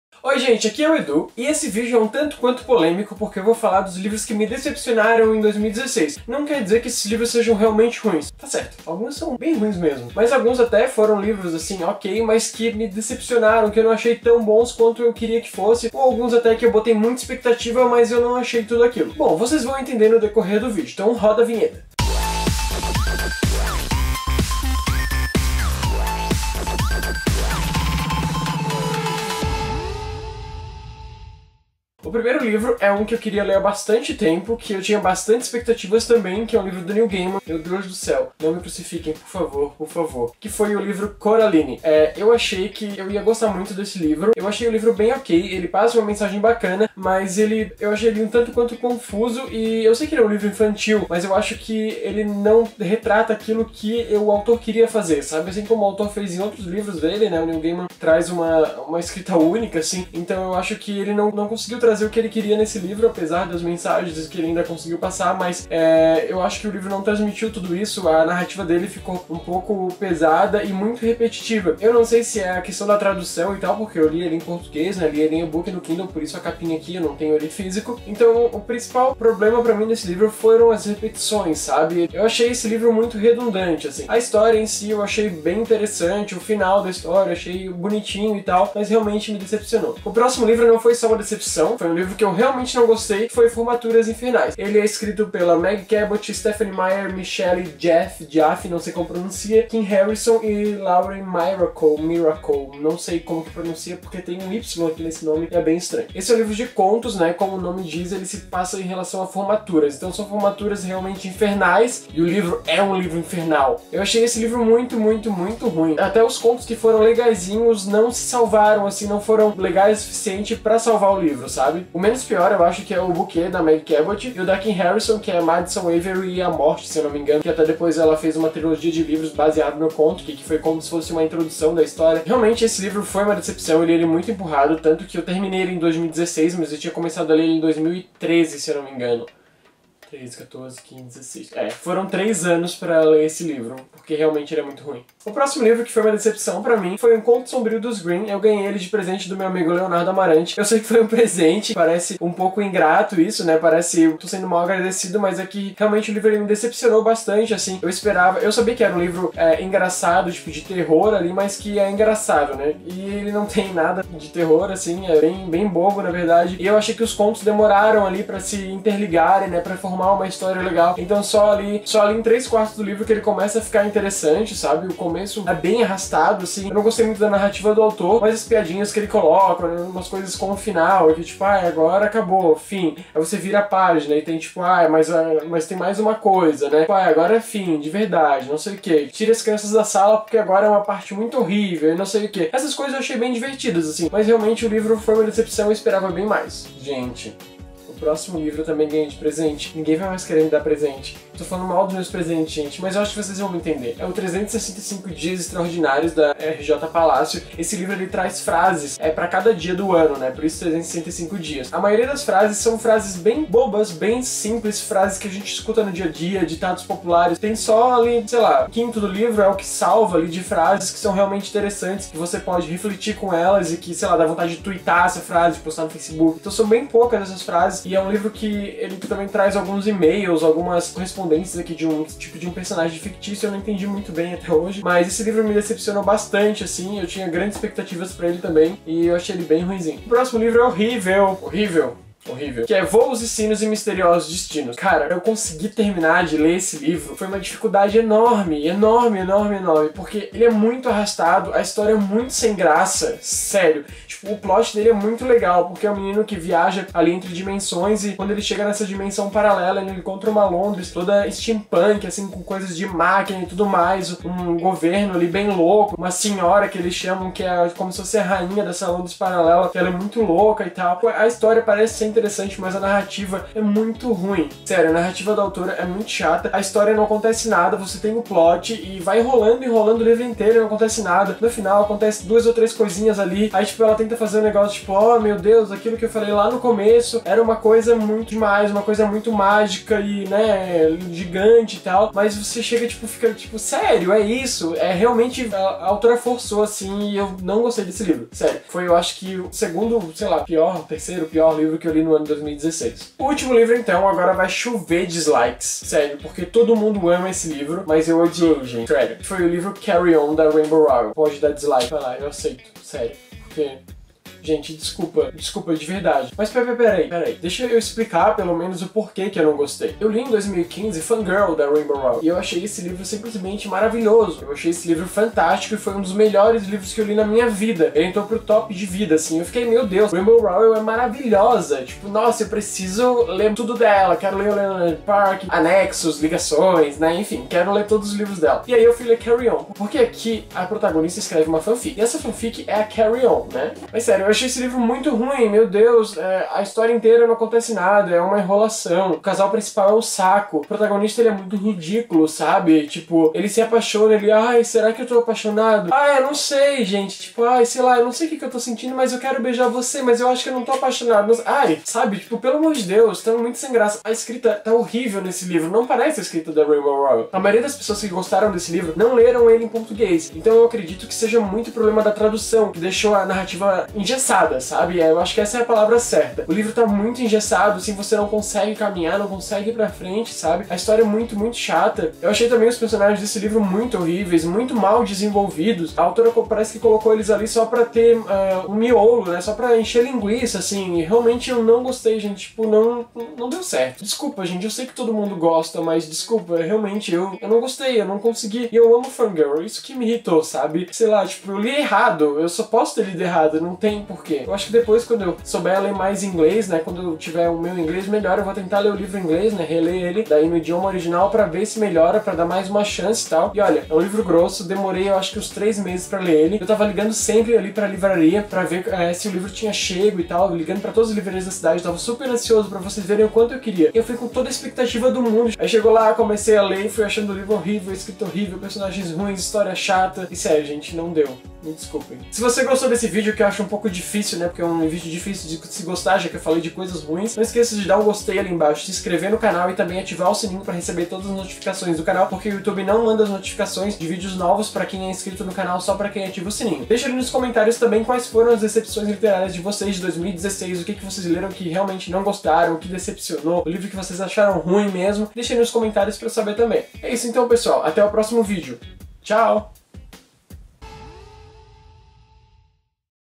DECEPÇÕES LITERÁRIAS DE 2016
Nem todas as leituras do ano foram boas... Nesse vídeo trago os livros que me decepcionaram no ano de 2016. Em alguns desses livros a leitura não valeu muito a pena...
COMPRE OS LIVROS CITADOS:
365 Dias Extraordinários
Formaturas Infernais
Voos e Sinos e Misteriosos Destinos
Um Conto Sombrio dos Grimm